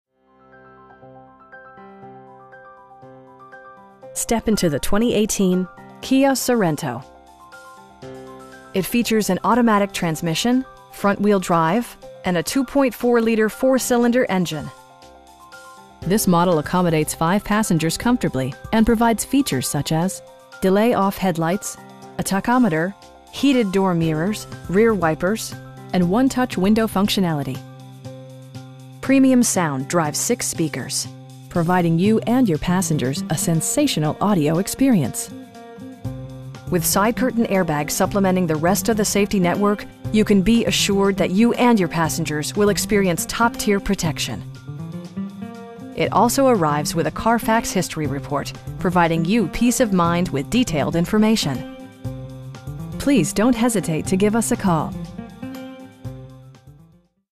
2018 Kia Sorento L FWD in Danbury, CT 06810-5014
Step into the 2018 Kia Sorento. It features an automatic transmission, front-wheel drive, and a 2.4 liter 4 cylinder engine. This model accommodates 5 passengers comfortably, and provides features such as: delay-off headlights, a tachometer, front bucket seats, heated door mirrors, power windows, rear wipers, and 1-touch window functionality. Premium sound drives 6 speakers, providing you and your passengers a sensational audio experience. With side curtain airbags supplementing the rest of the safety network, you can be assured that you and your passengers will experience top-tier protection. It also arrives with a Carfax history report, providing you peace of mind with detailed information. We pride ourselves on providing excellent customer service. Please don't hesitate to give us a call.